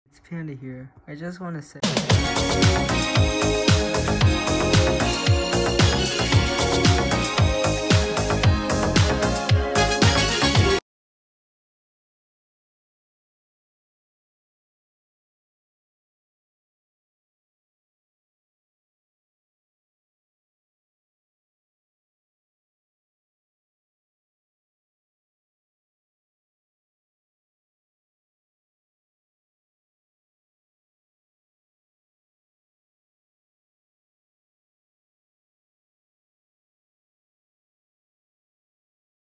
Deleting my Channel. + APOLOGY
Thank you for the great time I had on this channel.

Goodbye.